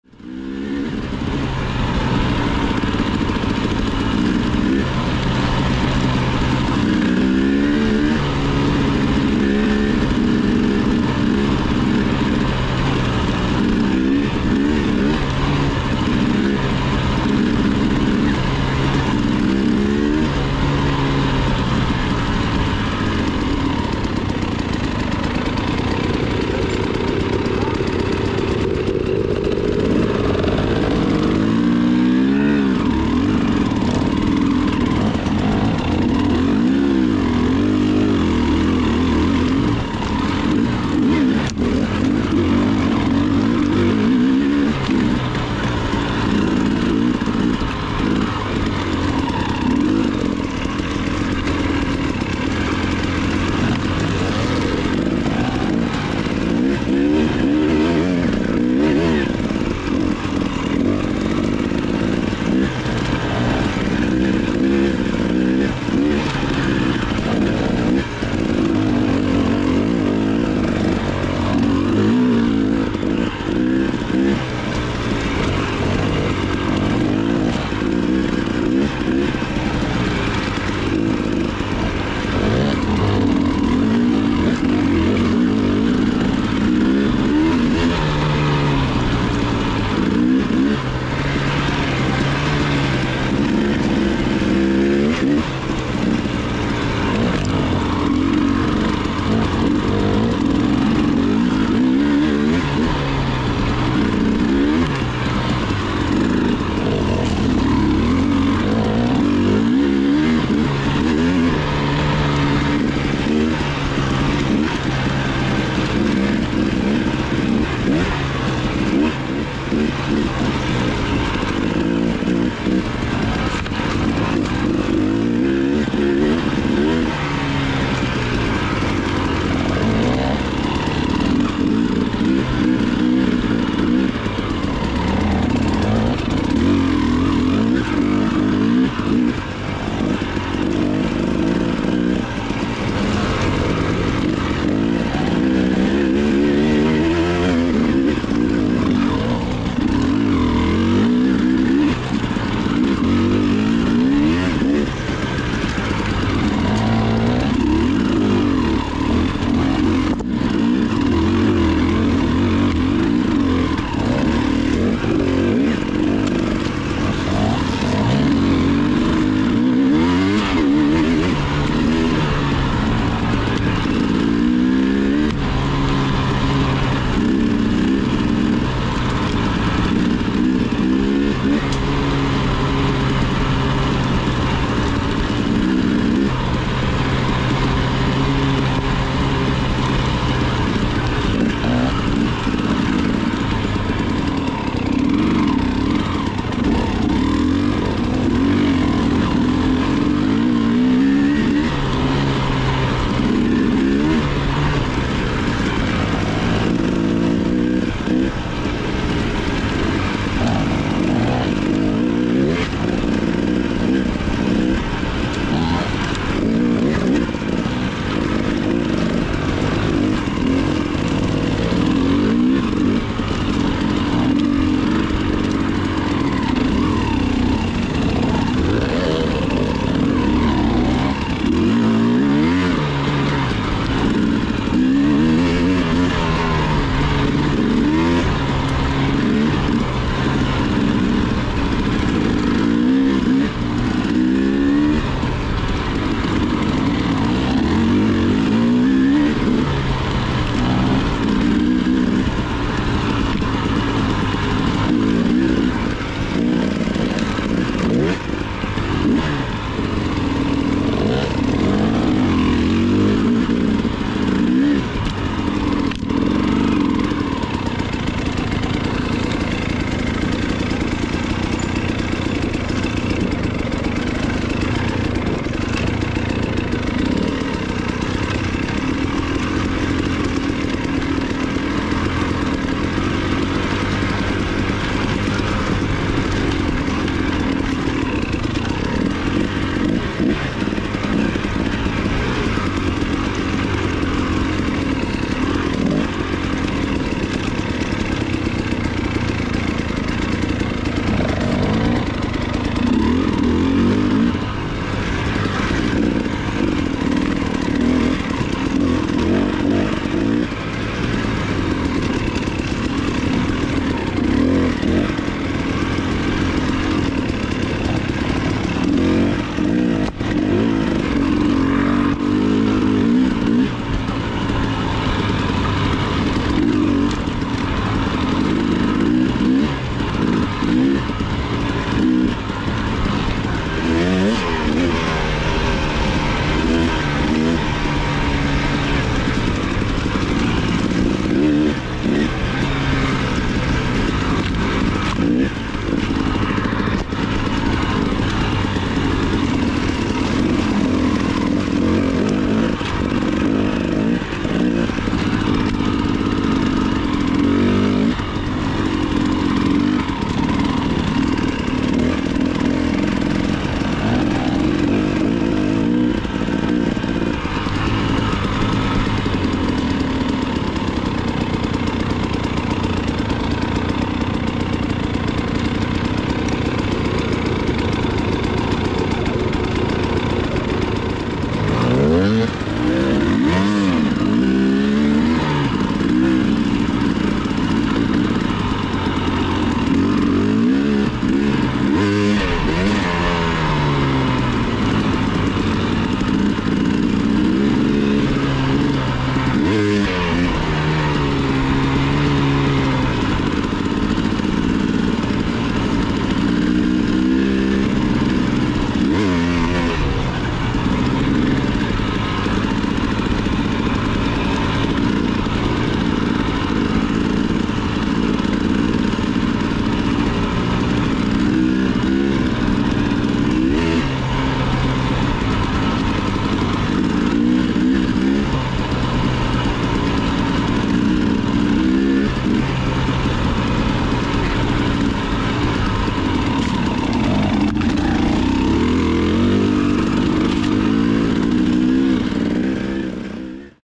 Single Track Night Ride - Cyclops 10.0 LED & Explorer 2 Light Bar
Some fun single track ripping with my buddies last summer.  I'm using lights from Cyclops Adventure Sports.  The main headlight is their 10.0 LED bulb with both Hi and Lo beam running.  Up top is their Explorer 2 LED bar.  This combination works really well and is very compact on the bike.  I've been using both of these lights for about 3 years now and they have held up very well.  No issues, highly recommend!

Video Record Date: August 20, 2022.